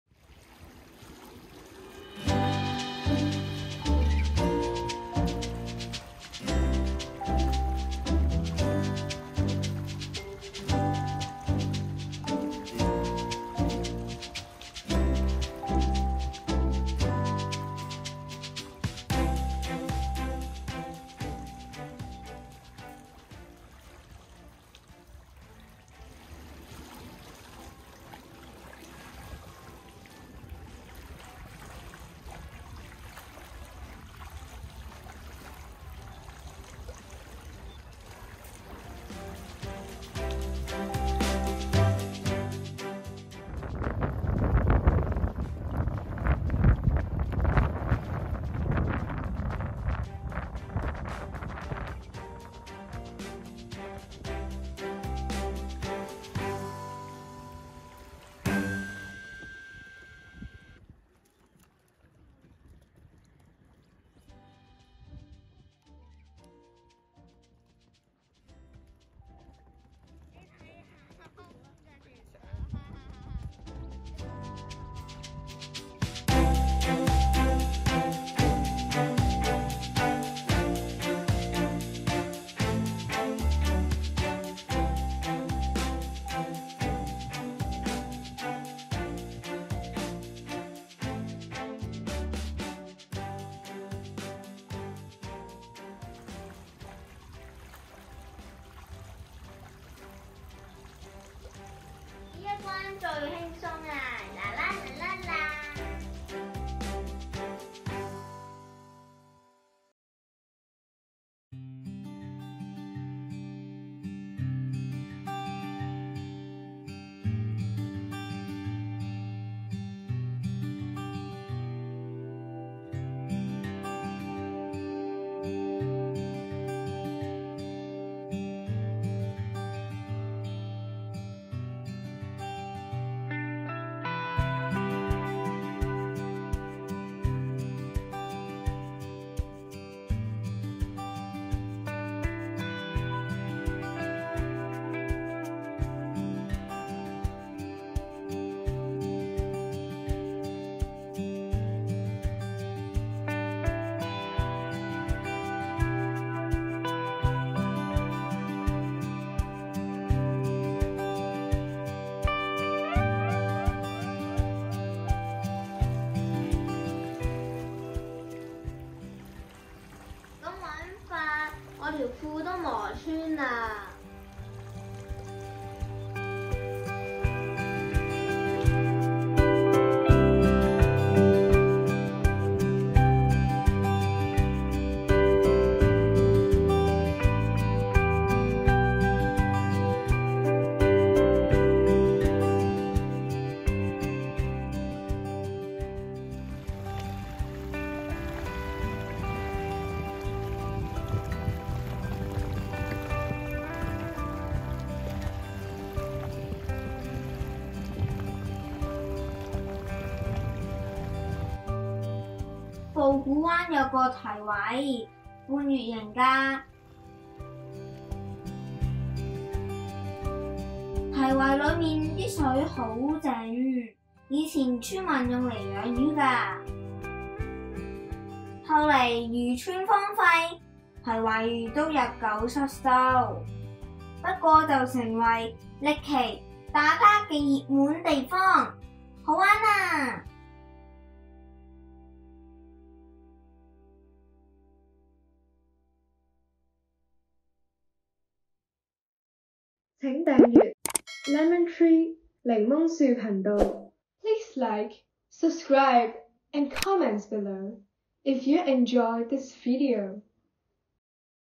山海歷奇完勝主題遊樂場丨曝罟灣過三關丨半月形堤圍丨上窰丨起子灣村丨學童在野丨自然教室丨漁村遺跡丨香港好靚丨Lemontree丨Hiking
新冠疫情第五波，學生提早放暑假，三月變初夏。

一於投奔大自然，戴住口罩也得呼吸青山綠水，無他，香港真係好靚！

去上窰，越起子灣村，下曝罟灣到達半月形堤圍，我們發現一種遺世美。

探看漁村遺跡，連四肢不勤的學童也開心到飛起！